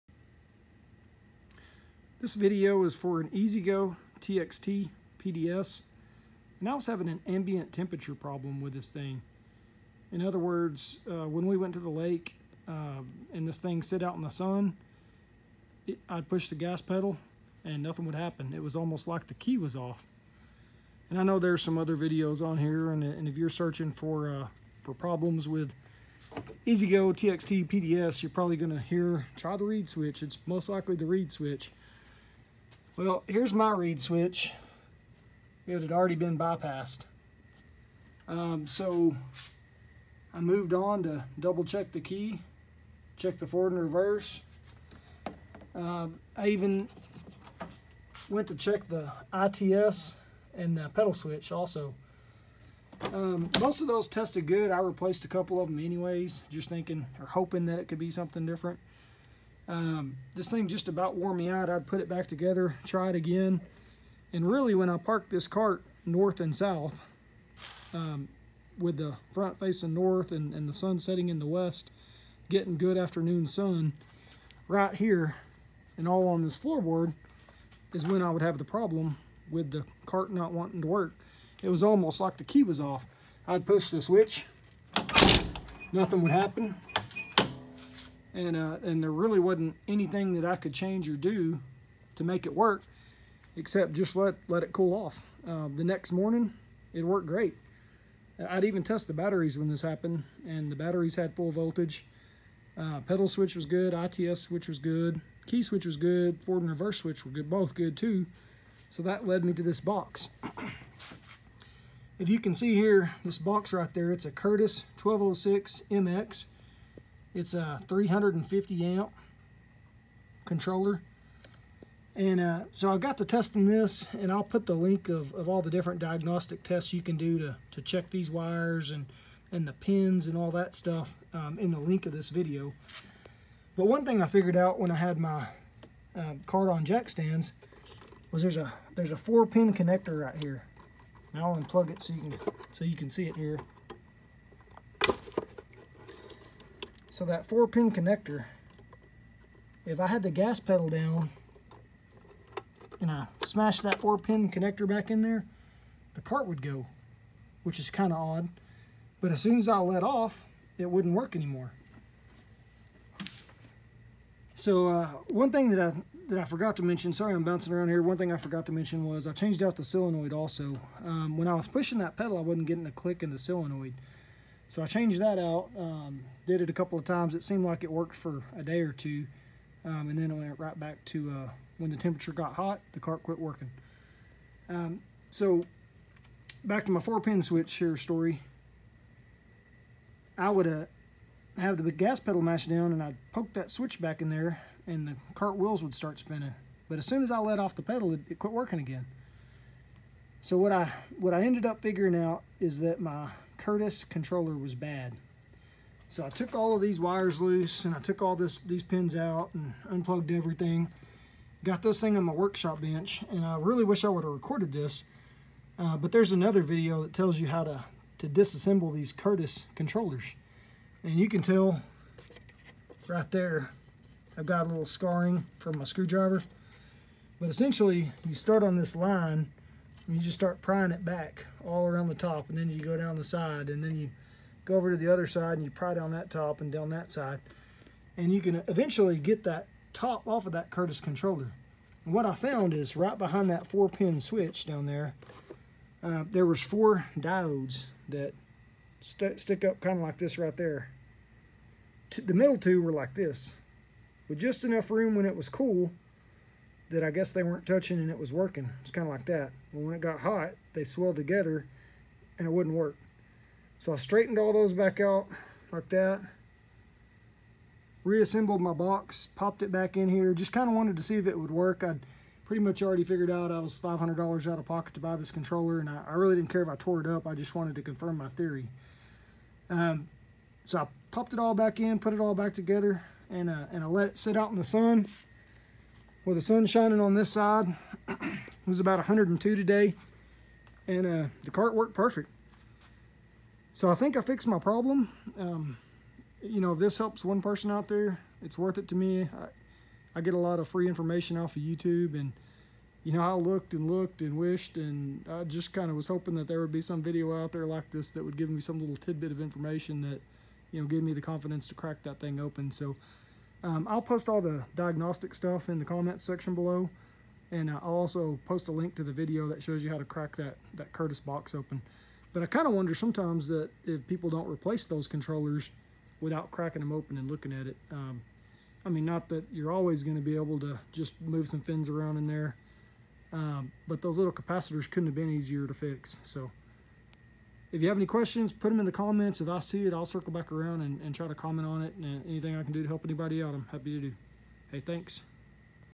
36 volt EZGO TXT won’t go when outside temp is hot.
EZGO TXT PDS ambient temperature problem solved!